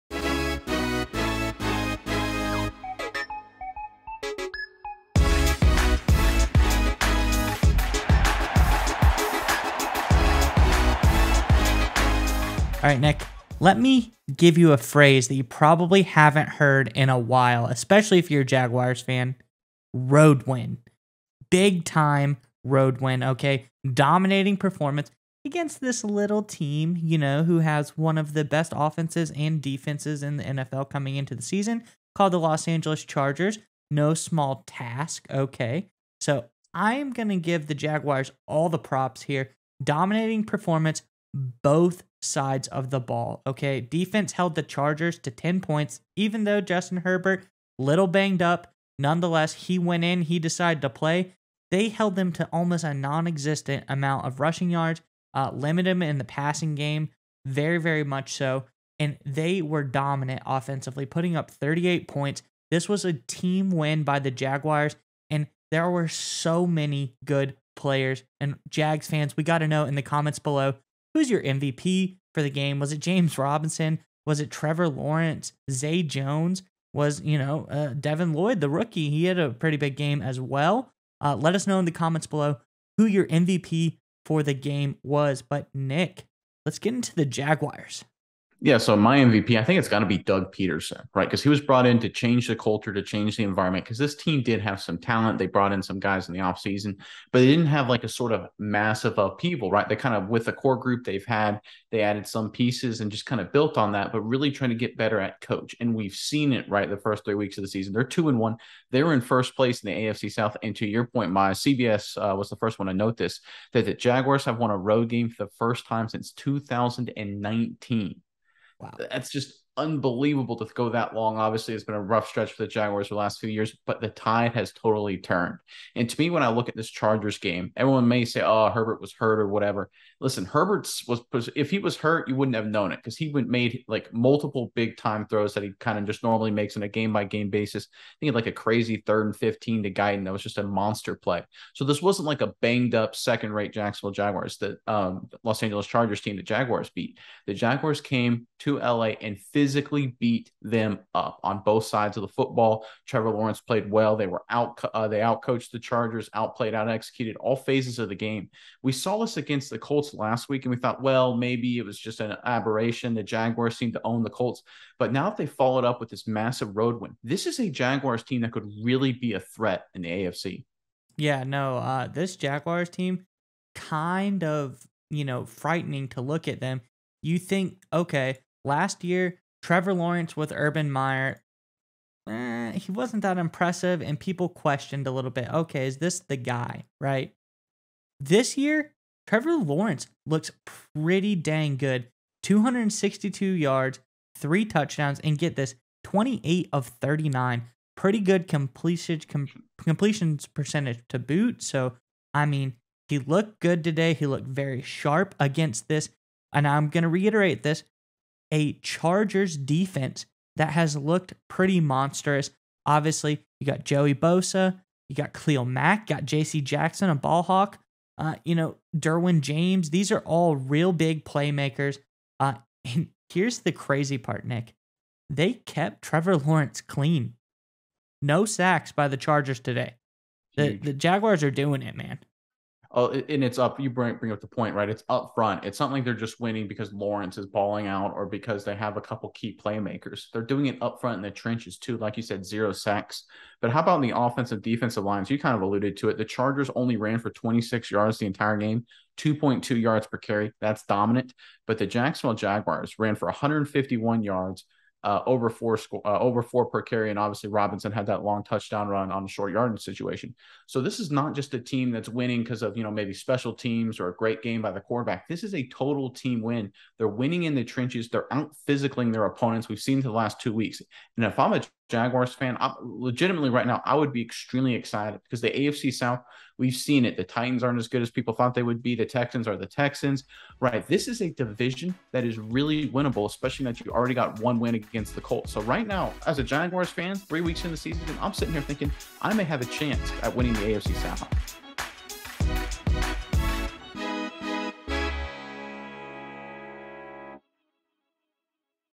Jaguars Have Become An Absolute THREAT
The Jacksonville Jaguars came off their big win last week with a lot of momentum. The Jaguars rode that momentum into this week with an even more impressive win. The Jaguars looked great in all phases of the game and were the more physical team. The Jacksonville Jaguars have become an legitimate contender.